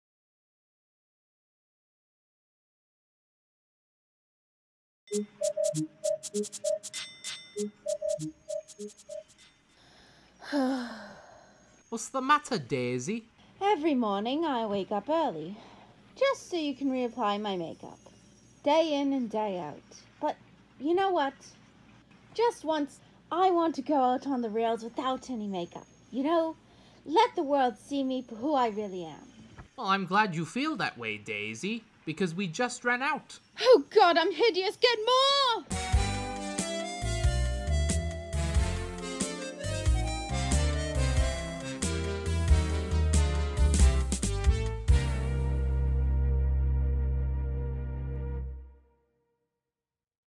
Daisy's Make Up
Daisy goes through her usual morning routine.

Please note this video is not intended for anyone under the age of 13 (10 at the youngest).